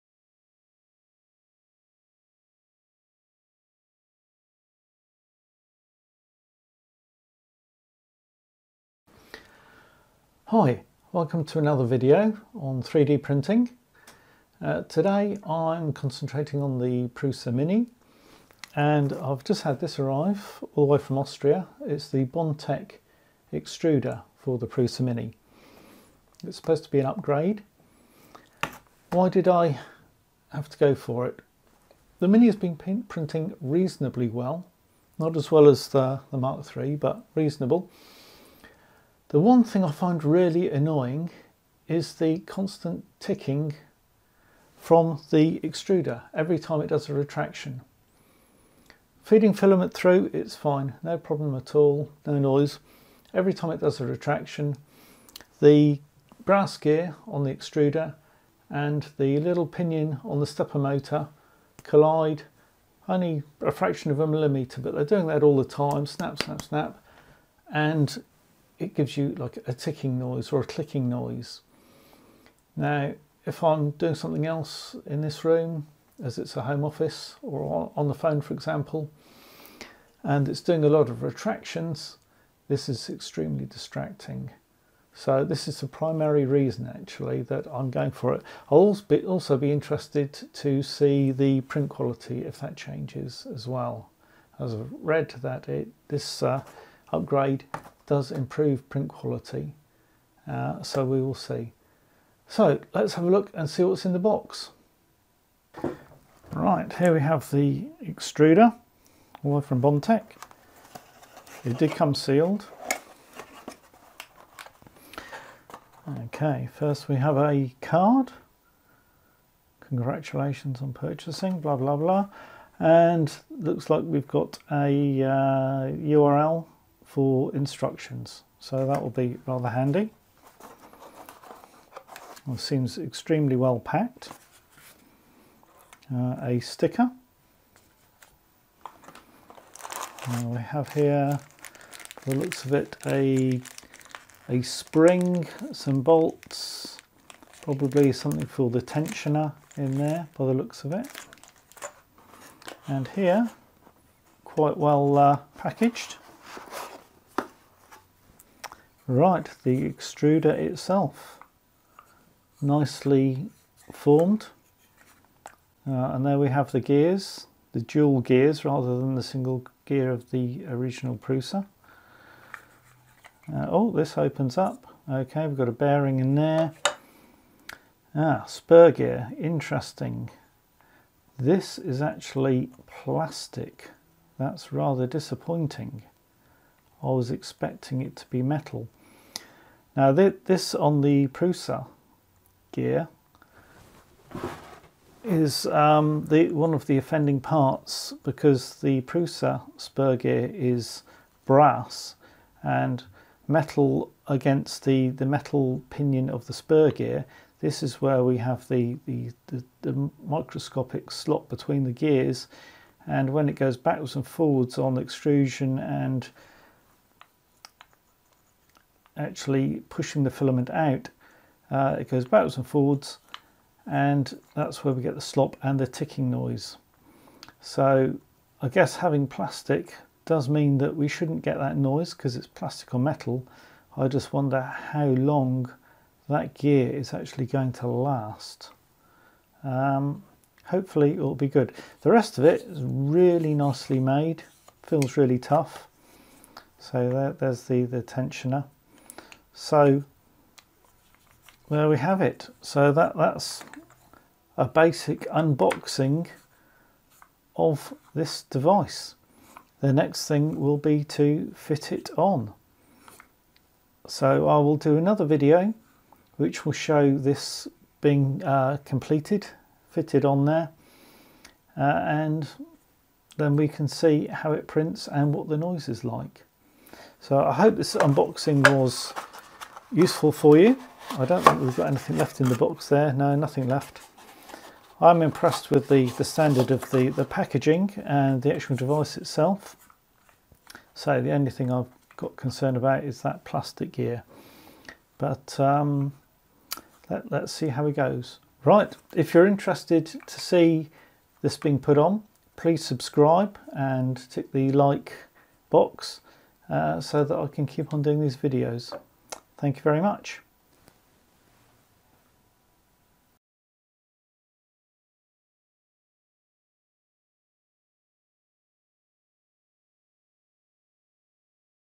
Prusa Mini Bondtech Extruder Unboxing (Part 1)
Part 1 - Unboxing of the Bondtech extruder upgrade for the Prusa Mini 3d Printer.
Future videos will include a review of anti vibration solutions for the Prusa Mk3/s and Mini.